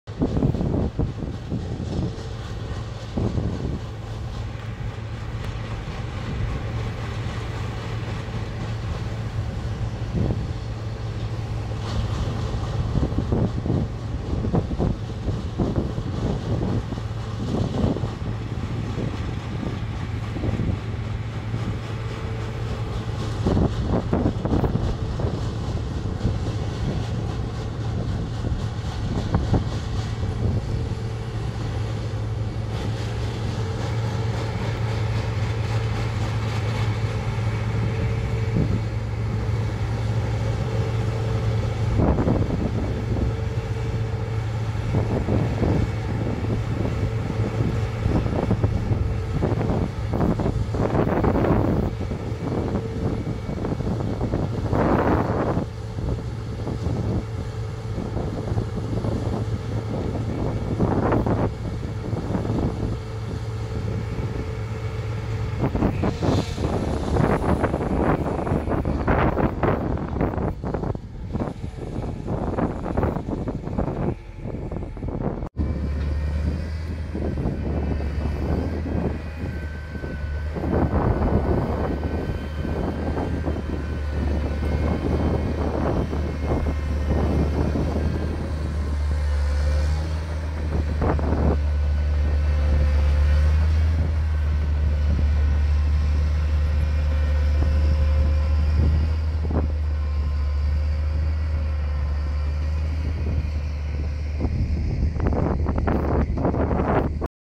John Deere CH570 Operators training at Phnom Penh Sugar
CH570 harvesting function working on the road 06 December 2016